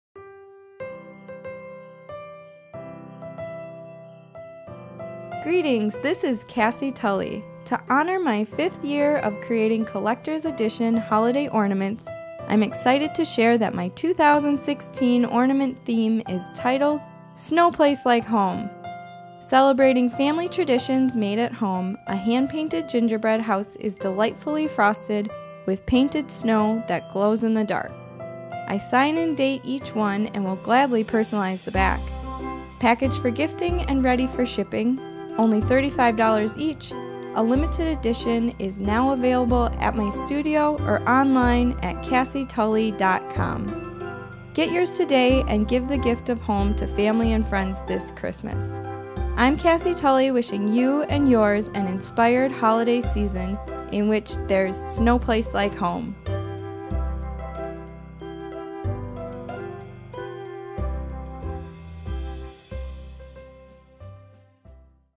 BEHIND THE SCENES Cassy Paints "Snowplace Like Home" Ornaments
"Snowplace Like Home," my 5th Anniversary Commemorative ornament captures the love of home with a charming gingerbread house. Thanks to your wonderful support, "Snowplace Like Home" ornaments are officially sold out. To order custom ornaments or select from my current collection of hand-painted ornaments, please